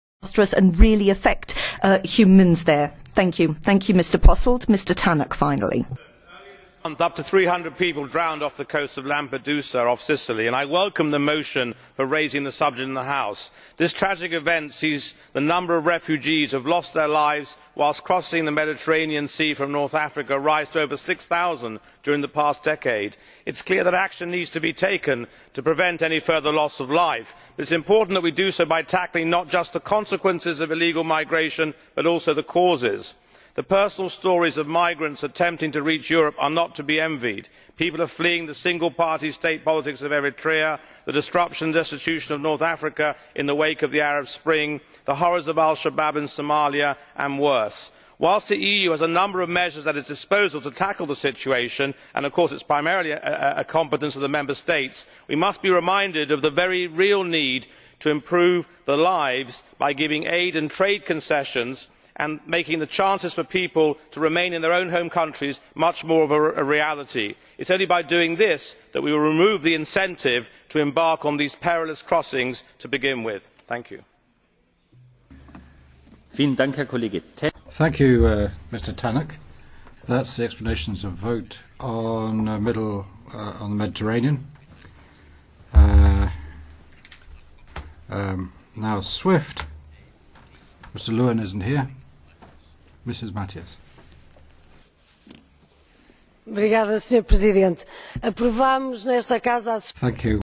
Lampedusa tragedy
Last month up to 300 people drowned off the coast of Lampedusa, off Sicily, and I welcome the motion for raising the subject in the House. This tragic event brings the number of refugees who have lost their lives whilst crossing the Mediterranean Sea from North Africa during the past decade to over 6 000.

It is clear that action needs to be taken to prevent any further loss of life, but it is important that we do so by tackling not just the consequences of illegal migration but also the causes. The personal stories of migrants attempting to reach Europe are not to be envied. People are fleeing the single-party-state politics of Eritrea, the destruction and destitution of North Africa in the wake of the Arab Spring, the horrors of Al-Shabaab in Somalia, and worse. Whilst the EU has a number of measures at its disposal to tackle the situation -- and of course this is primarily a competence of the Member States -- we must be reminded of the very real need to improve lives by giving aid and trade concessions and making the chance for people to remain in their own home countries much more of a reality. It is only by doing this that we will remove the incentive for people to embark on these perilous crossings in the first place.